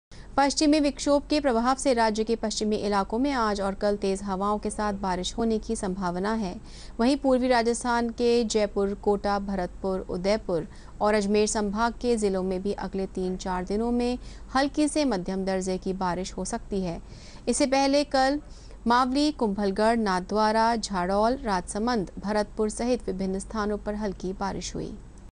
राज्य के पश्चिमी इलाकों में आज और कल तेज हवाओं के साथ बारिश होने की संभावना
- पश्चिमी विक्षोभ के प्रभाव से राज्य के पश्चिमी इलाकों में आज और कल तेज हवाओं के साथ बारिश होने की संभावना है। वहीं पूर्वी राजस्थान के जयपुर, कोटा, भरतपुर, उदयपुर और अजमेर संभाग के जिलों में भी अगले तीन चार दिन में हल्की से मध्यम दर्ज की बारिश हो सकती है।